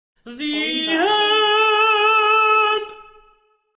Sound - "The End" sung, ascending, by Opera Man (long) | AudioJungle
Download Sound - "The End" sung, ascending, by Opera Man (long) | AudioJungle

A singer friend of mine with an operatic, dramatic voice, singing "The End," ascending (long release). Check my portfolio for the full Opera Man collection.